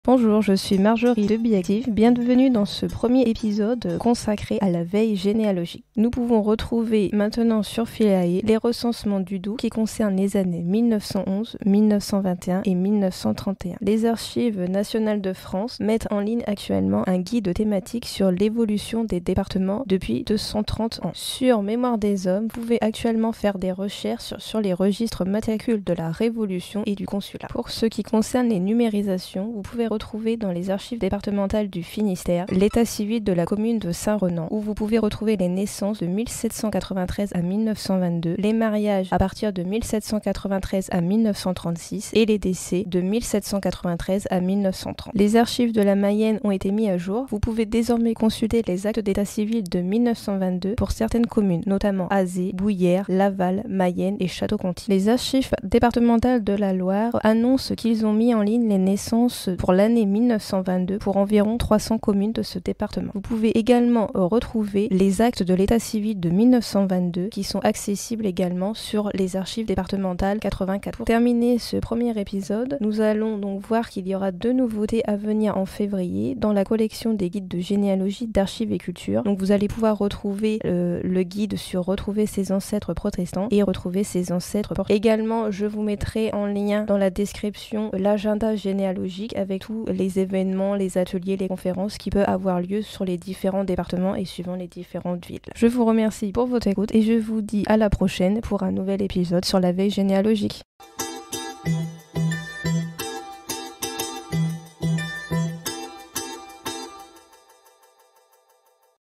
#1. La veille généalogique
Bienvenue dans ce premier épisode consacré à la veille généalogique !

Ce que tu vas découvrir :
✨ Les actes d'état civils et les recensements mis en ligne depuis le 12 janvier 2023
✨ Les fonds divers mis en ligne
✨ Les différentes parutions (livres, vidéos...)
✨ Les évènements généalogiques à venir